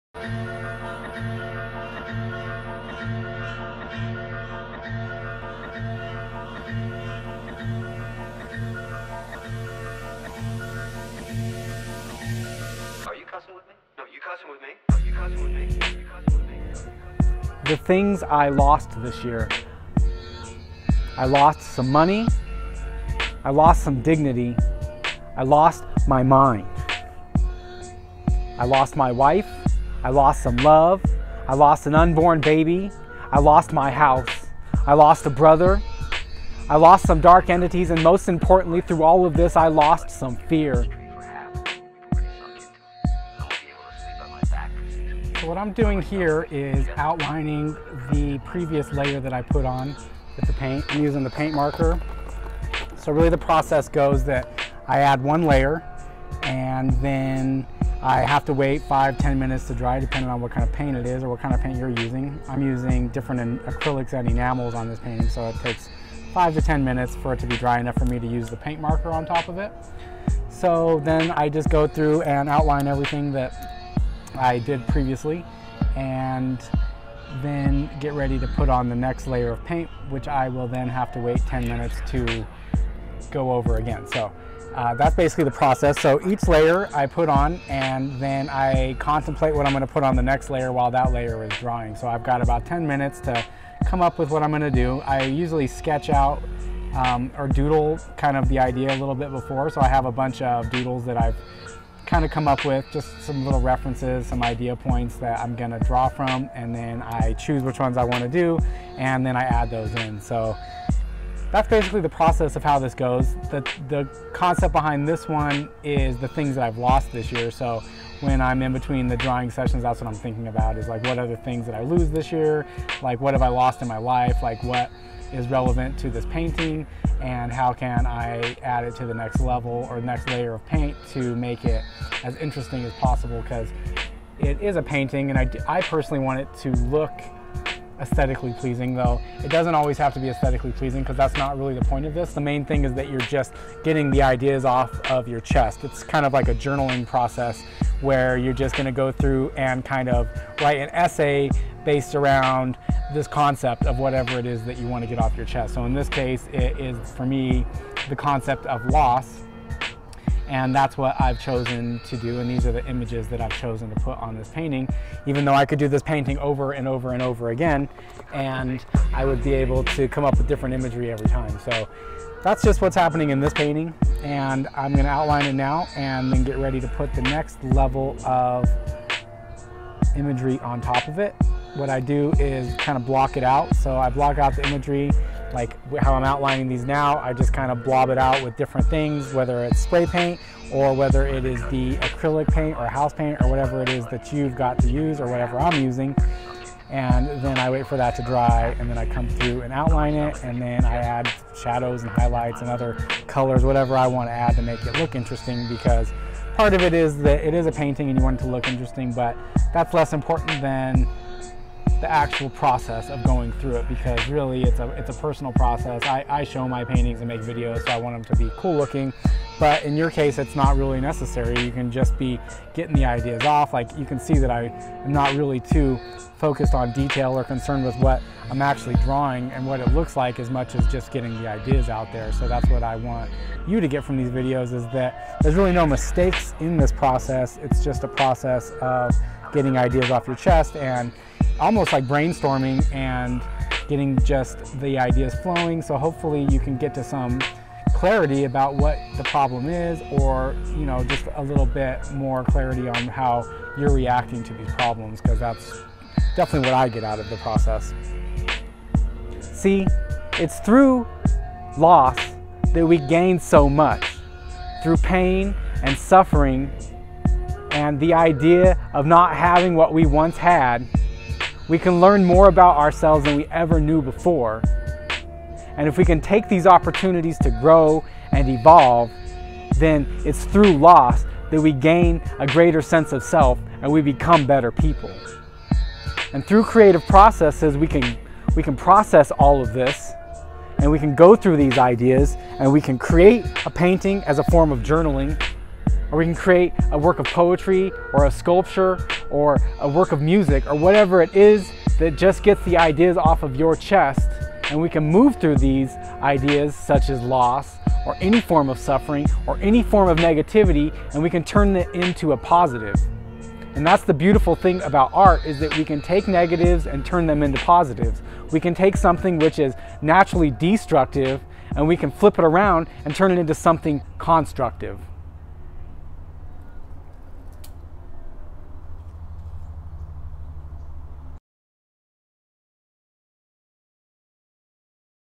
The Shadows Episode 9 The Things I Lost Matte Refic
in this video, I confront the issue of loss and how it affects our lives and how to turn those same perceptions into art.